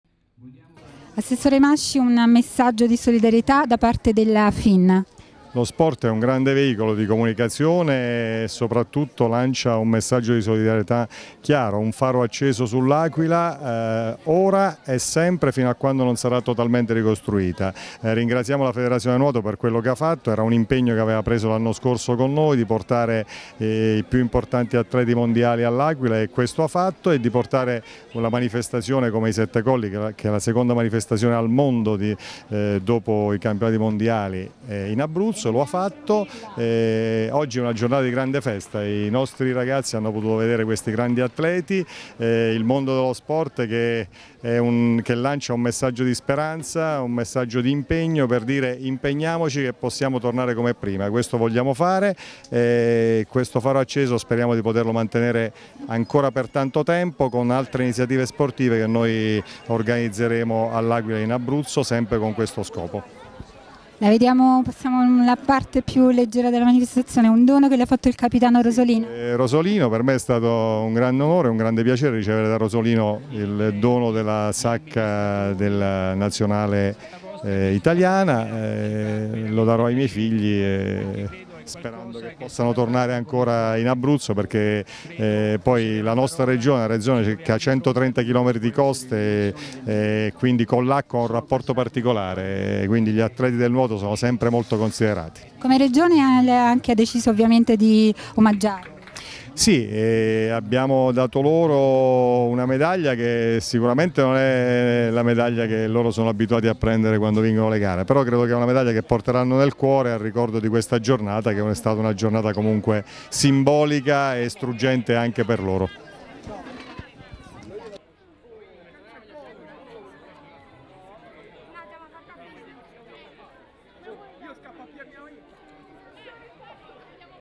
Ass. Masci Incontra gli atleti italiani della FIN
SPORT; MASCI:FIN E ATLETI A L'AQUILA PER NON DIMENTICARE
LA SOLIDARIETA' DELLE AUTORITA' LOCALI E PROVINCIALI
(REGFLASH) - L'Aquila, 15 giu. Il Presidente della FIN -Roma, Senatore Paolo Barelli ha evidenziato con la presenza a L'Aquila "la volontà di non dimenticare la vicenda del terremoto", il Presidente della Provincia di L'Aquila, Antonio Del Corvo ha ribadito "l'impegno istituzionale affinchè L'Aquila ed i comuni del 'cratere' abbiano un'attenzione diversa data la situazione" ed ha ringraziato gli sportivi Fin; il Sindaco di L'Aquila Massimo Cialente ha esortato i giovani a "copiare la forza dei campioni di nuoto per rinascere" ed ha ricordato che grazie ad ESSO Italia nel 2009 sono state recuperate due piscine danneggaite dal sisma; Aurelio Cilli, assessore allo Sport della Provincia di Pescara ha sottolineato la disponibilità totale da parte dell'amministrazione provinciale ad ospitare l'evento. Tra le autorità che hanno preso parte alla premiazione il Prefetto di L'Aquila Giovanna Iurato. Un ringraziamento è stato espresso dai presenti, tra i quali il vice sindaco Arduini, ad Ermanno Morelli, seduto tra gli atleti, Presidente del CONI Regionale Abruzzo, ed a Michele Castaldi per il lavoro svolto come Protezione Civile, organizzatore della visita in zona rossa. L'incontro con le autorità ecclesiastiche e l'Arcivescovo e la cerimonia in piscina è stato organizzato da Filomena Ibello della Direzione Attività Sportive della Regione, dalla FIN-FIN CR Abruzzo - società RN L'Aquila, di cui è Presidente Giovanni Benevieri. Prima dell'inizio della cerimonia i campioni si sono fermati con i bimbi allievi delle società di nuoto per la firma degli autografi e le foto. Rosolino ha omaggiato l'assessore Masci con una borsone sportivo della nazionale di nuoto.(REGFLASH) A.S. 100615